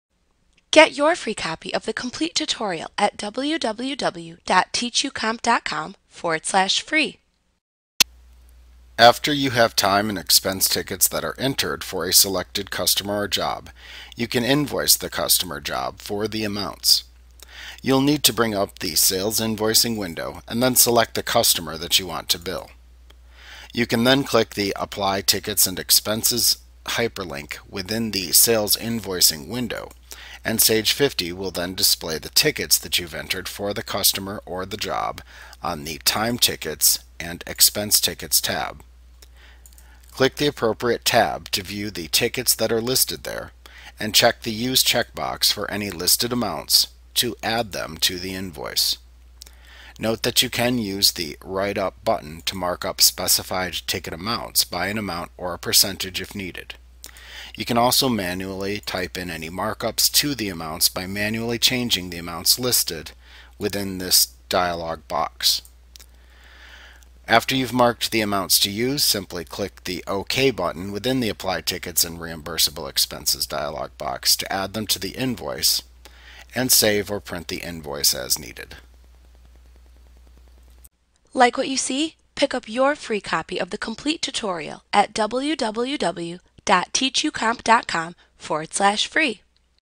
Sage 50 2014 Tutorial Billing Time and Expense Tickets Sage Training Lesson 12.6
Learn about billing time and expense tickets in Sage 50 A clip from Mastering Sage 50 Made Easy v. 2014. - the most comprehensive Sage 50 tutorial available. Visit us today!